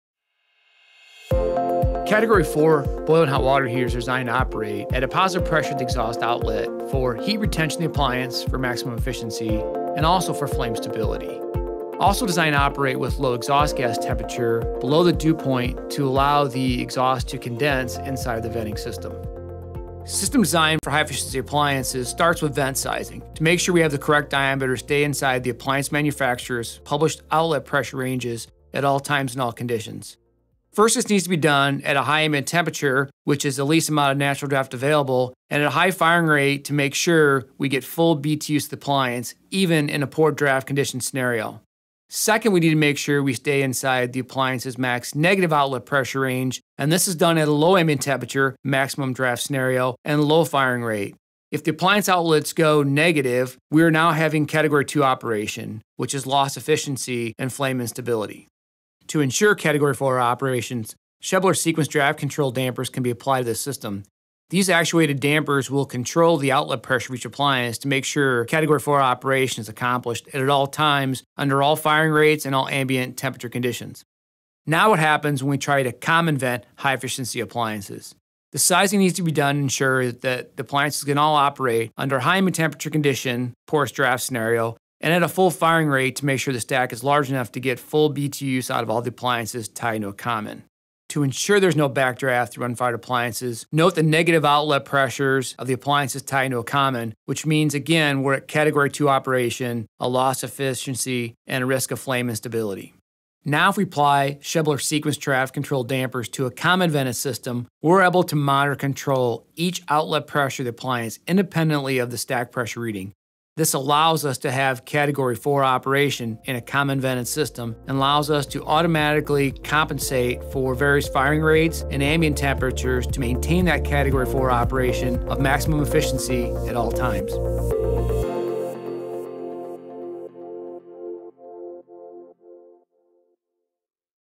High Efficiency Boiler Exhaust Venting | Schebler Chimney
Interested in learning how our Sequence Draft Control™ system can improve boiler and/or hot water heater performance and efficiency?